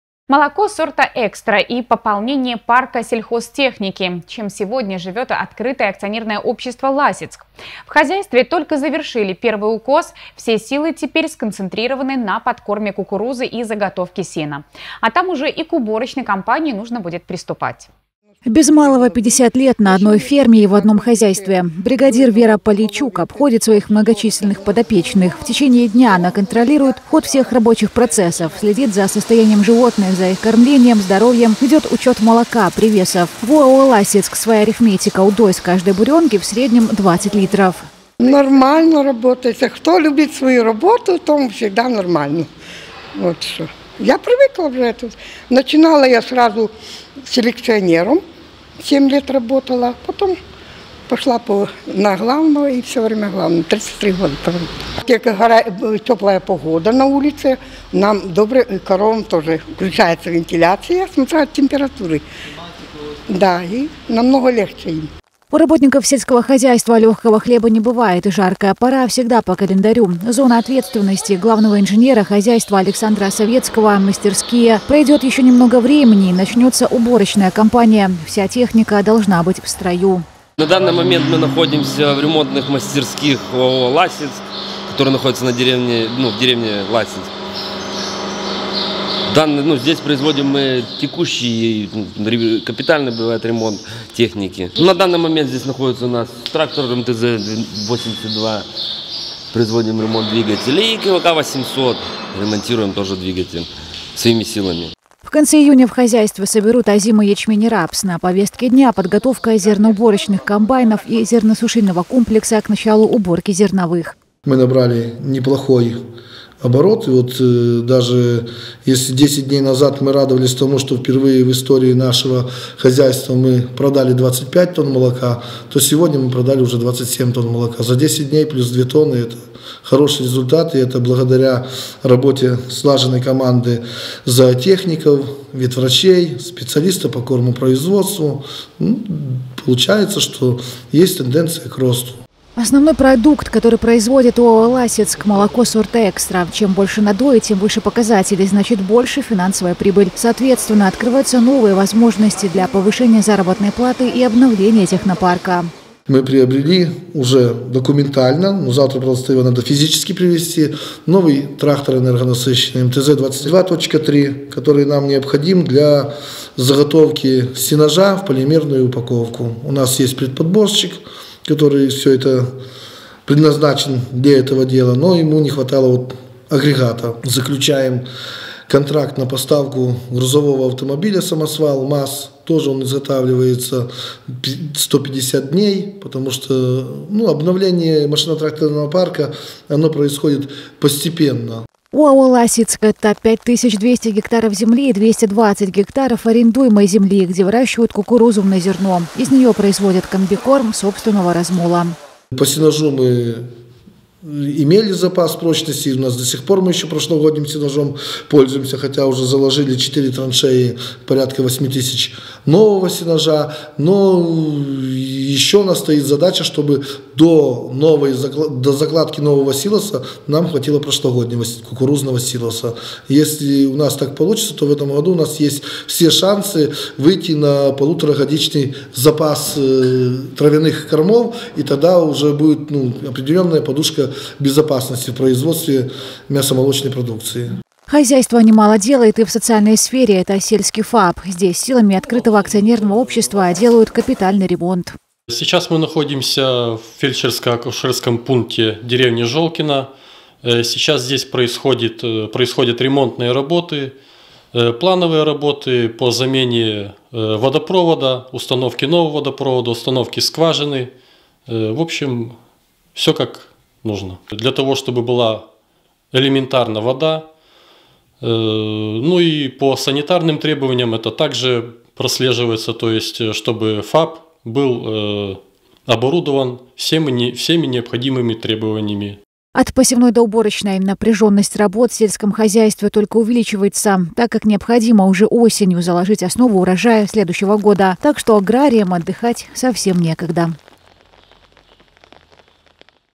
Молоко сорта "экстра": ОАО "Ласицк" наращивает темпы производства
Молоко сорта "экстра" и пополнение парка сельхозтехники: чем сегодня живет открытое акционерное общество "Ласицк?". В хозяйстве только завершили первый укос, все силы теперь сконцентрированы на подкормке кукурузы и заготовке сена. А там уже и к уборочной кампании нужно будет приступать.